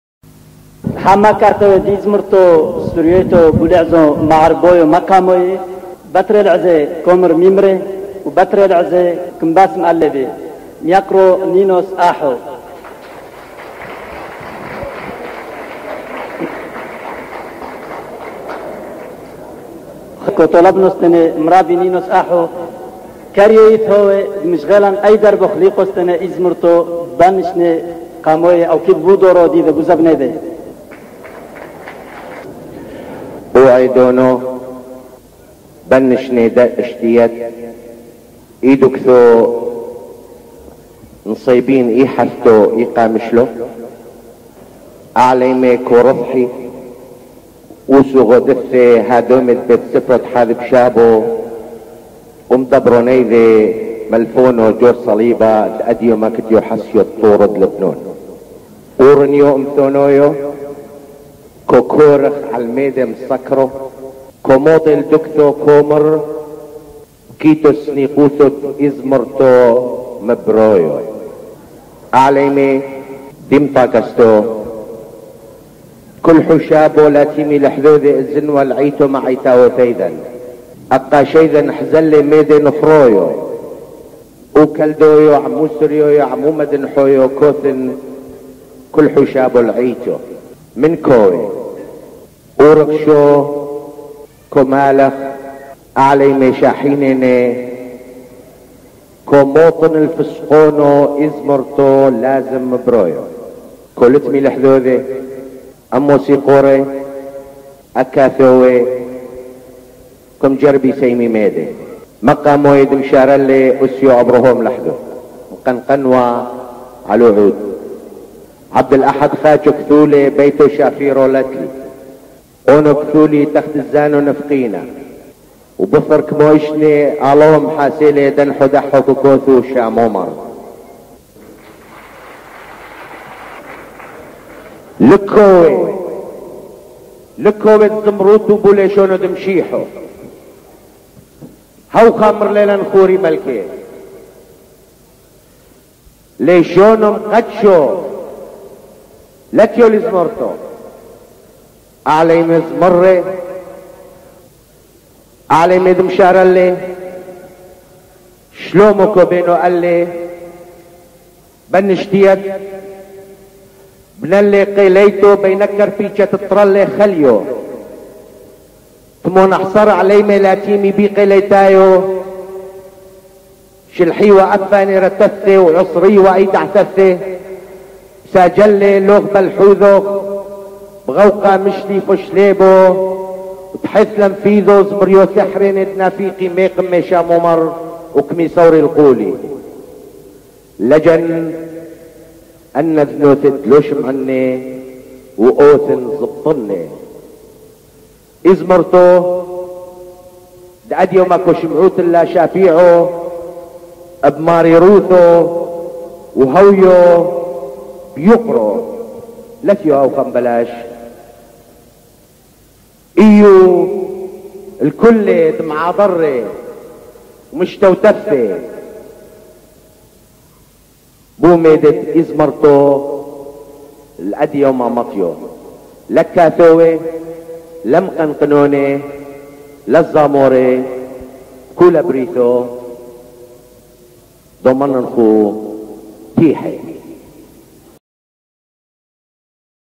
06 Syriac Folk Songs 1967 - Speech Ninos Aho
Speech Ninos Aho in Bruxelles

Moderation: Nail beth Kinne